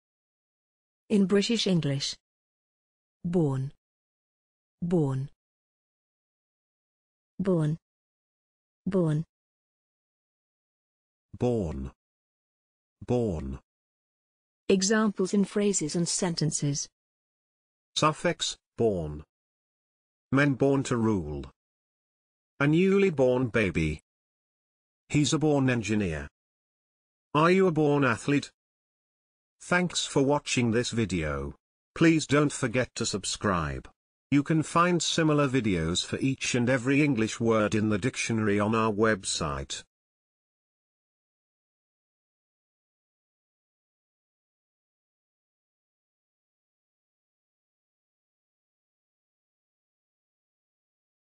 born - pronunciation + Examples in sentences and phrases
This is the pronunciation of born in British English + a few examples of its pronunciation in phrases and sentences.